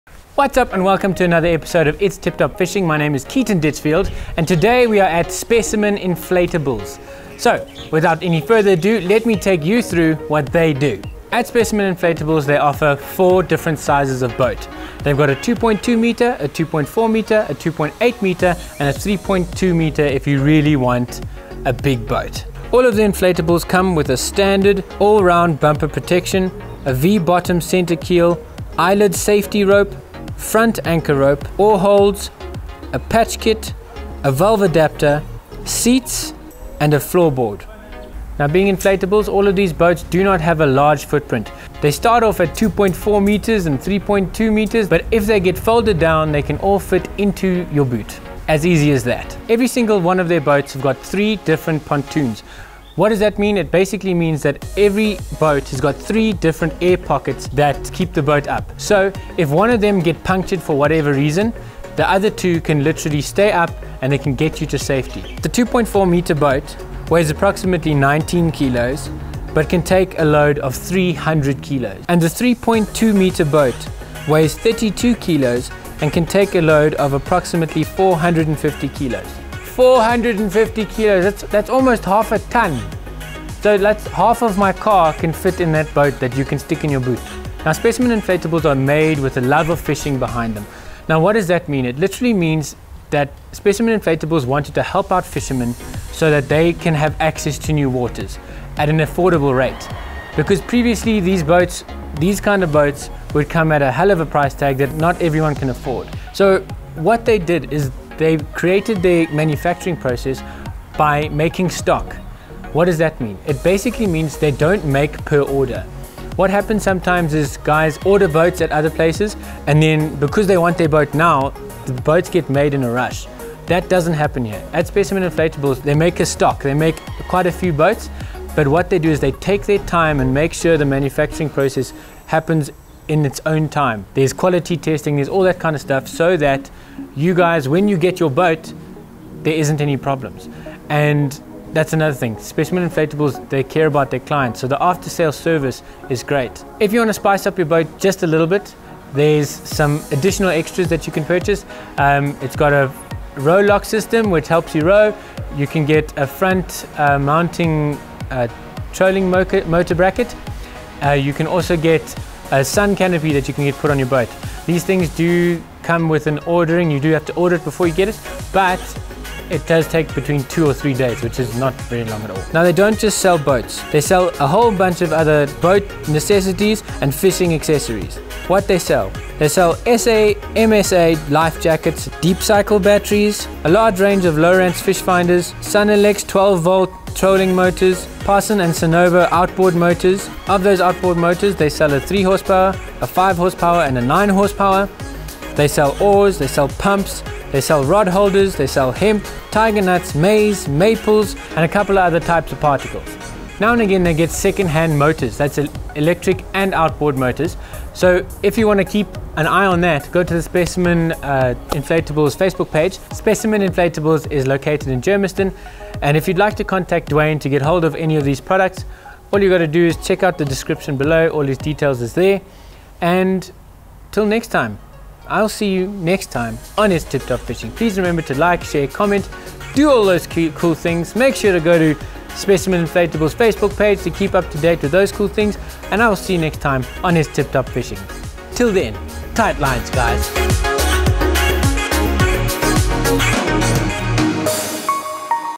Specimen Inflatables Headquarters
In this video, Keaton takes you through the headquarters of Specimen Inflatables and goes through what they have to offer, a few manufacturing details, and a few of the extra things that Specimen Inflatables does for their customers.

Thanks so much for your valuable time!